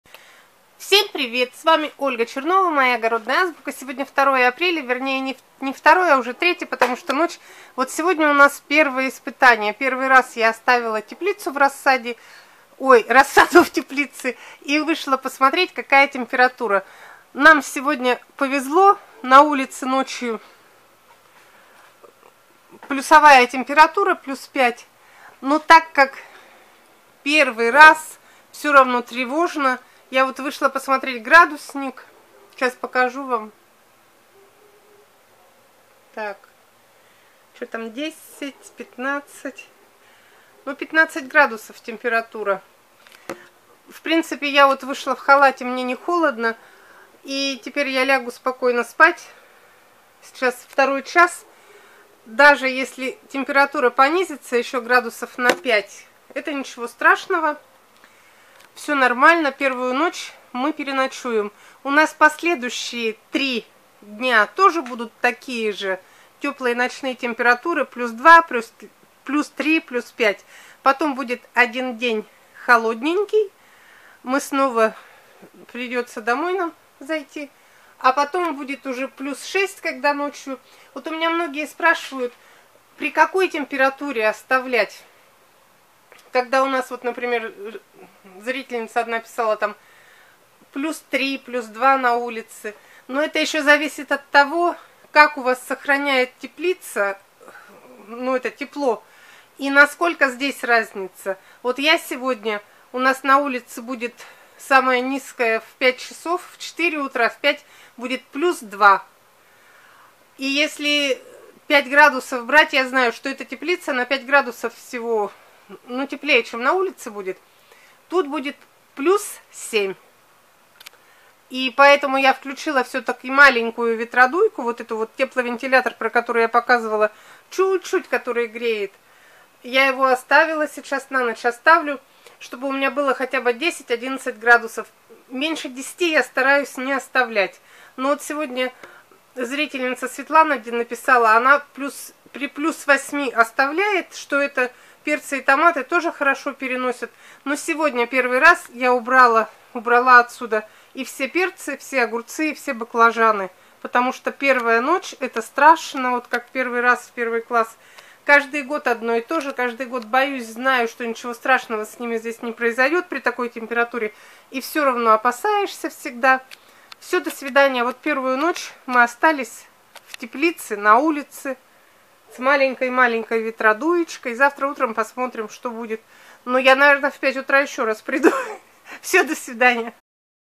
ПЕРВАЯ НОЧЬ В ХОЛОДНОЙ ТЕПЛИЦЕ. Ольга Чернова.
Сегодня первый раз оставила всю свою рассаду на ночь в теплице. Это тревожное ночное видео о том, какая температура у меня в теплице в 2 часа ночи и как я волнуюсь каждую весну в первую ночь.